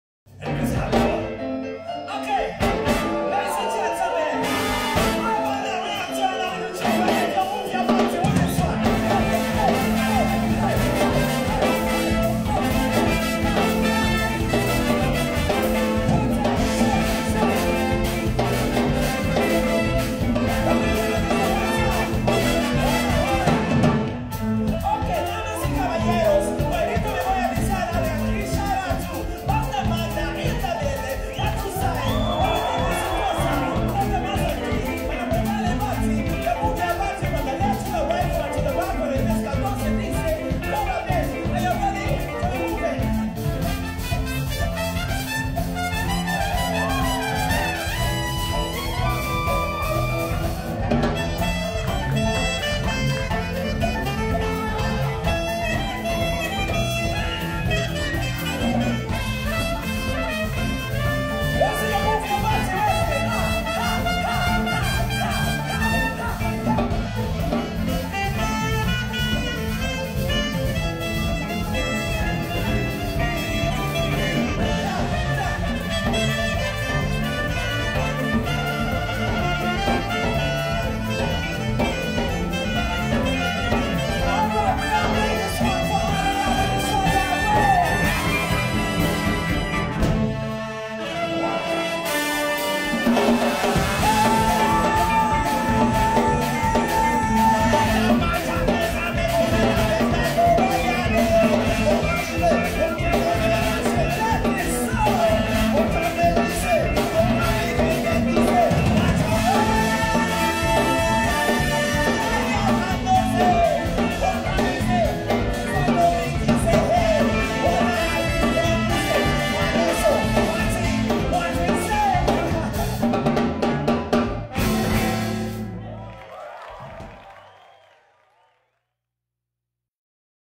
Banda Magda In Residence Project Impression
An Impression of the Banda Magda In Residence Project. Thanks Clemens Horn for the footage and Lara Frank and Simon Müller for the editing.